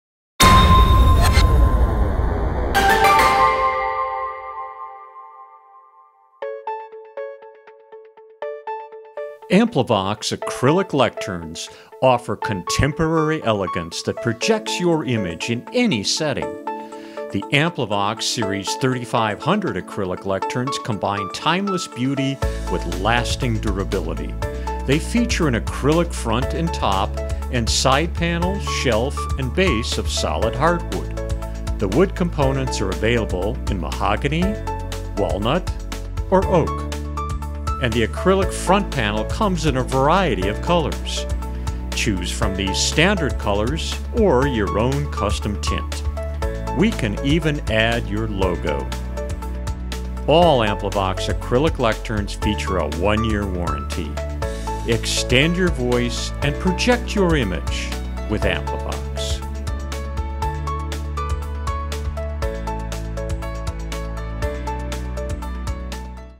3500 Wood & Acrylic Floor Lectern Podium Series
AmpliVox Acrylic Lecterns offer contemporary elegance that projects your image in any setting.

The AmpliVox Series 3500 Acrylic Lecterns combine timeless beauty with lasting durability. They feature an acrylic front and top and side panel, shelf and base of solid hardwood.

The wood components are available in Mahogany, Walnut, or Medium Oak. And the acrylic front panel comes in a variety of colors. Choose from these standard colors, or your own custom tint.

We can even add your logo. All AmpliVox Acrylic Lecterns feature a one year warranty.

Extend your voice and project your image with AmpliVox.